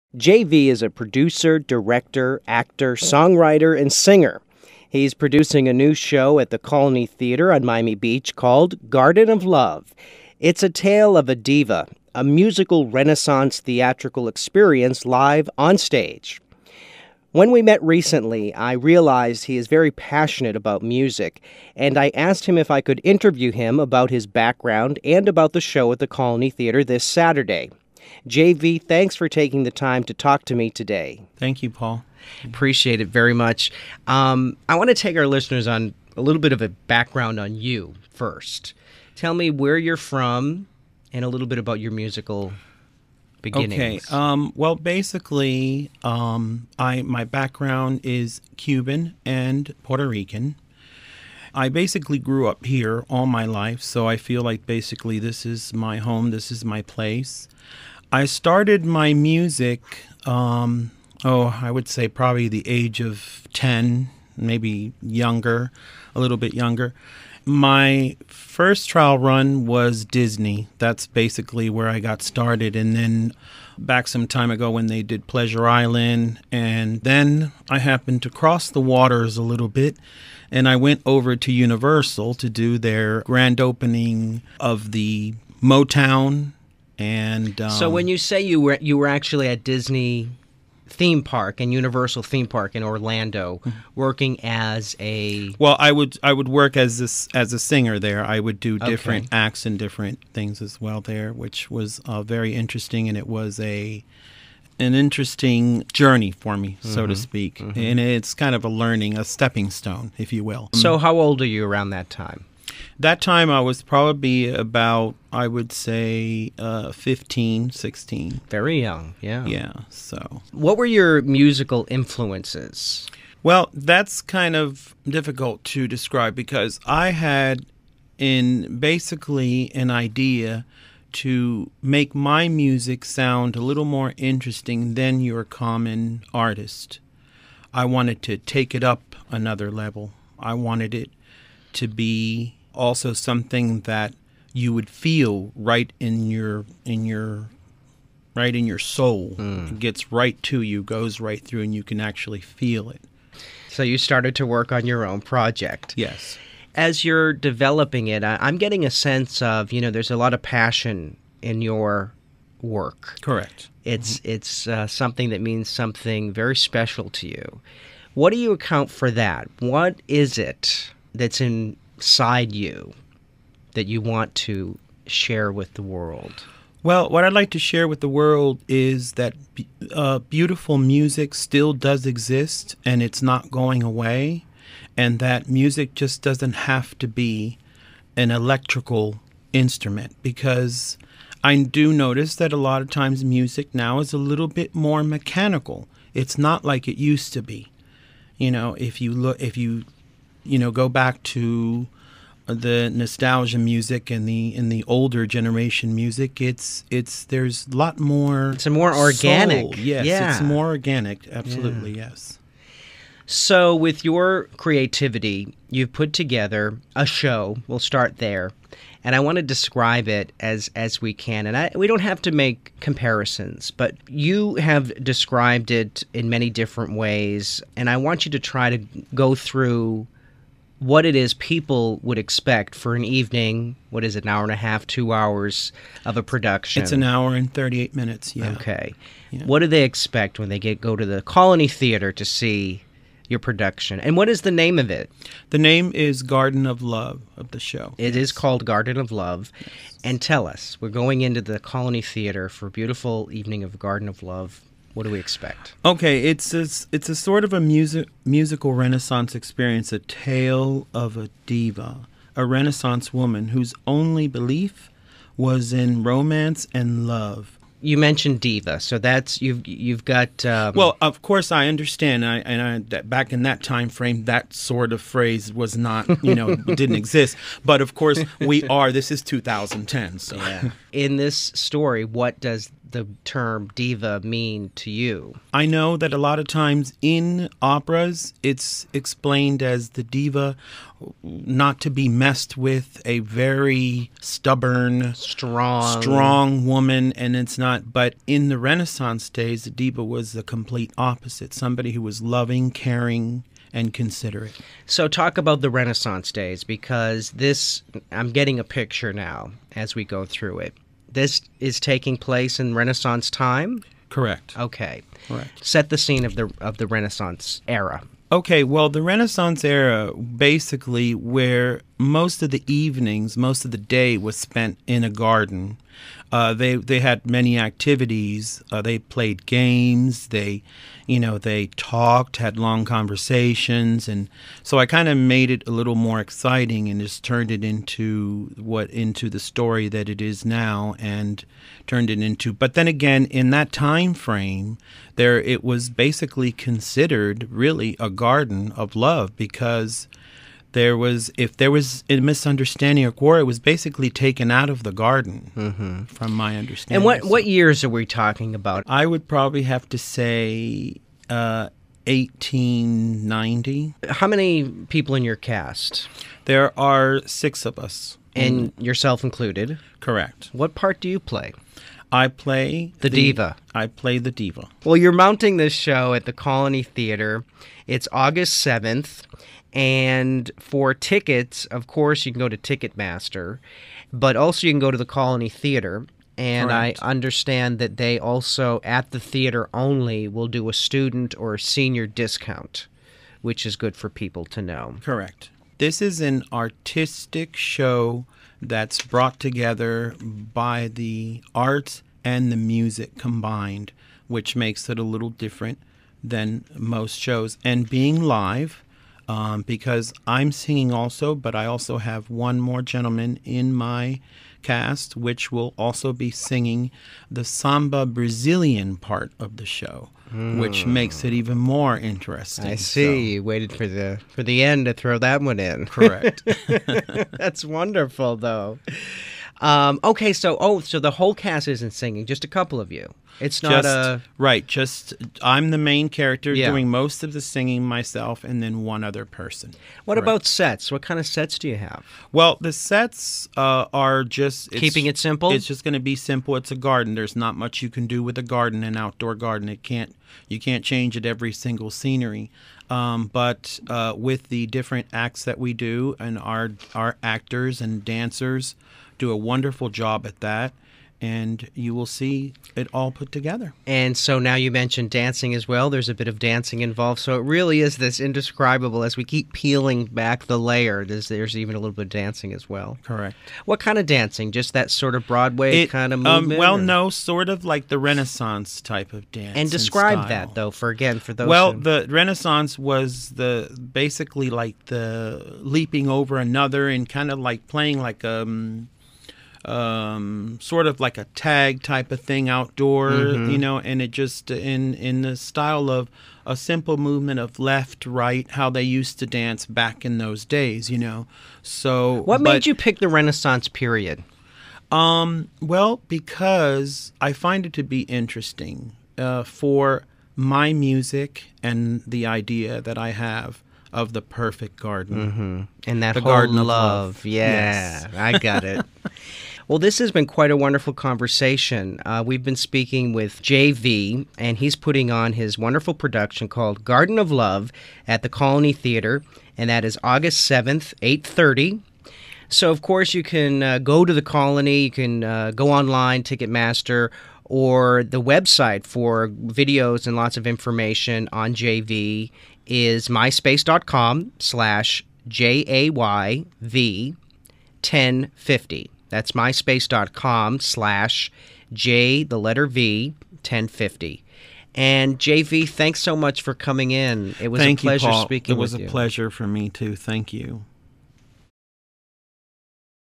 Garden of Love A Musical Renaissance Theatrical Experience
Paul Leary interviews Jay Vee the creator of Garden of Love the tale of a Diva and A Musical Renaissance Theatrical Experience at the Colony Theatre Miami Beach Saturday, August 7, 8:30pm.

FROM JAY VEE "Garden of Love has been canceled we will reschedule."